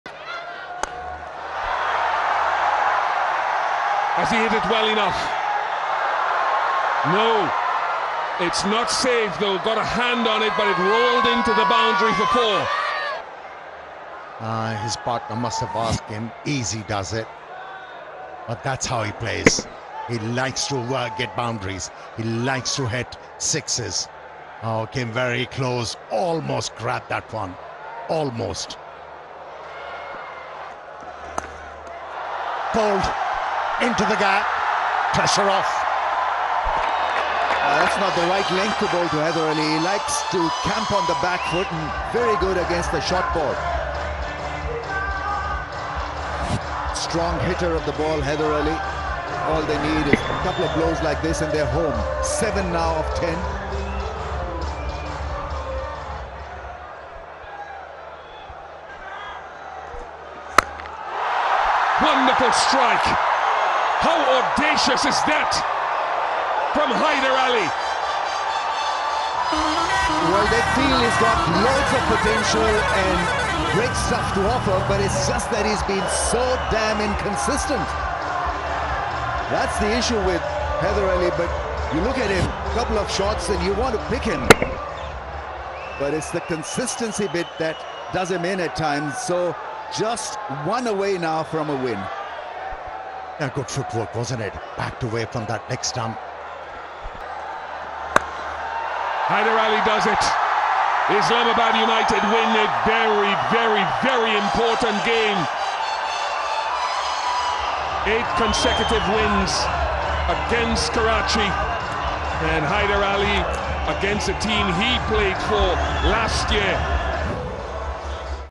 PSL 9 | Haider Ali! Take a Bow | What a Comeback | Islamabad United vs Karachi Kings | M1Z2A
PSL 9 | Haider Ali! Take a Bow | What a Comeback | Islamabad United vs Karachi Kings | Match 24 | M1Z2A